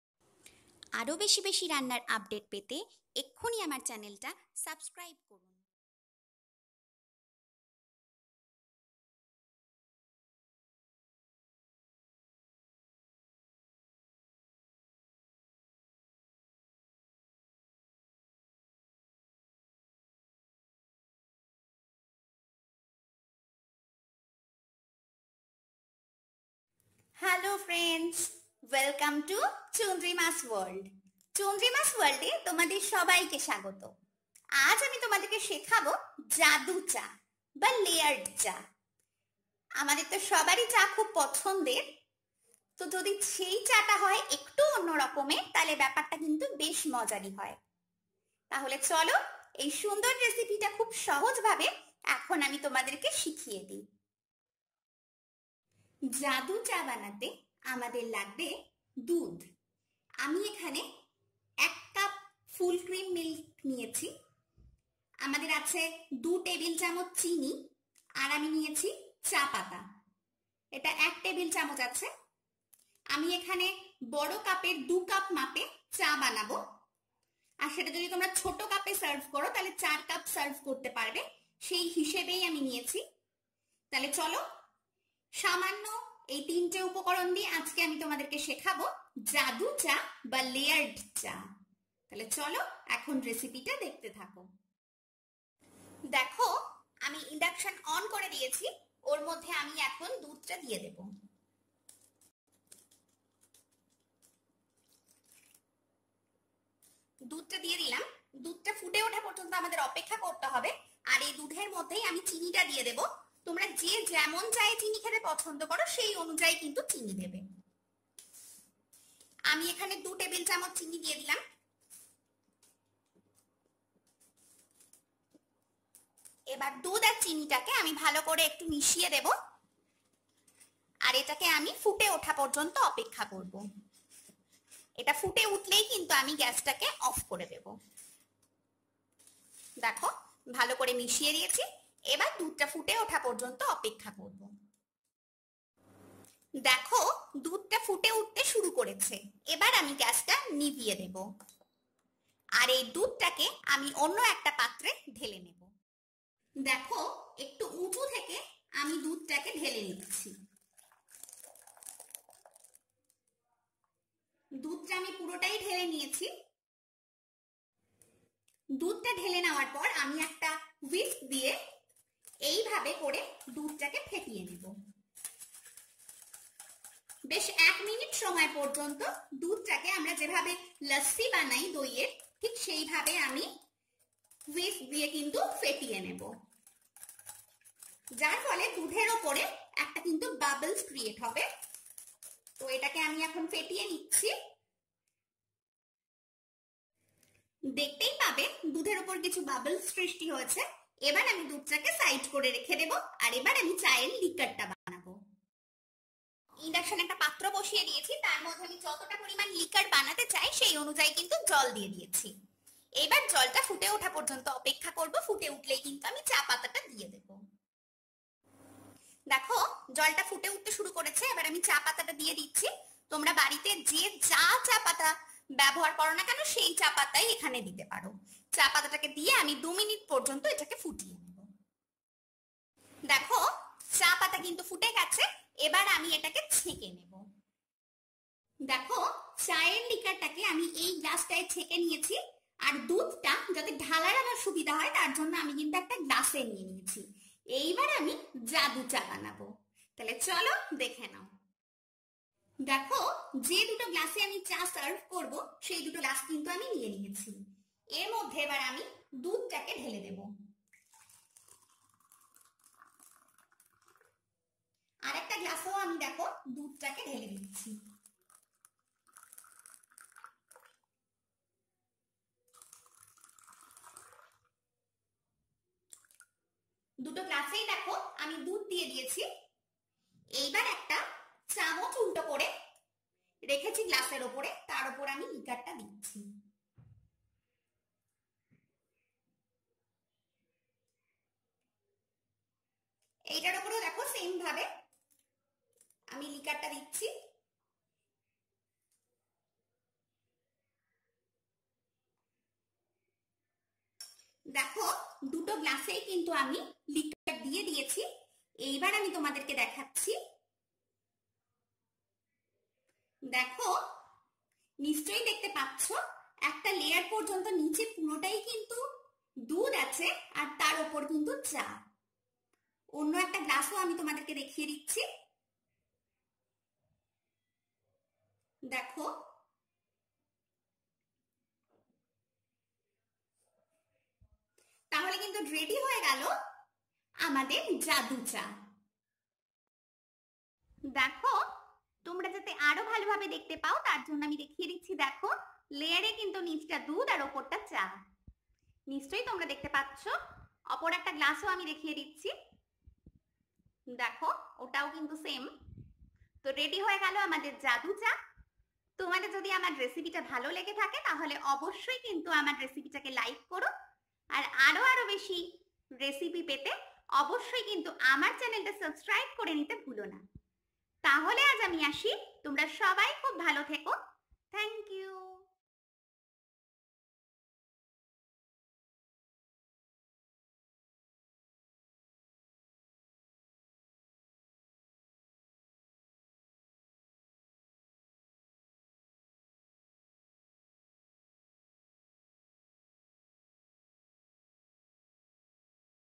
জাদু চা - Magic Tea Making At Home - Layered Tea
Here is the recipe how to make magic tea at home very easily, this is a refreshing tea recipe, please try this recipe and your family members love to drink this tea.

Layered milk tea
Magic tea
Jadu cha
Innovative milk tea
Layer milk tea making at home
Chandrima's world